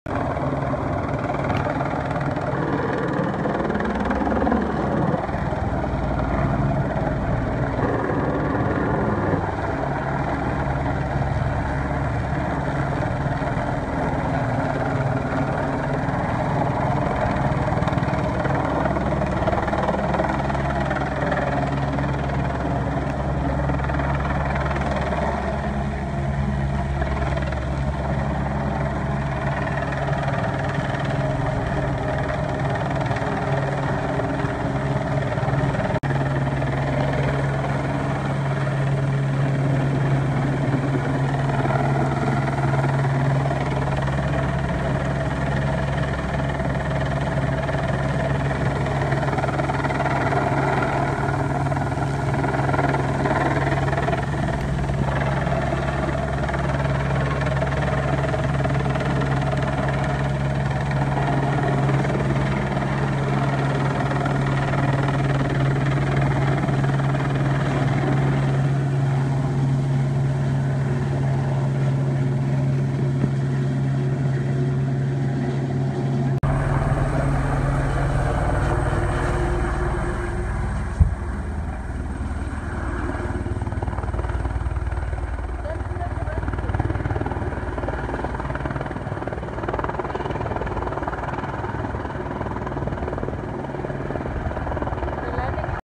Helicopter dropping wood for foot bridges
Trail crew replacing foot bridges at Monkman Cascade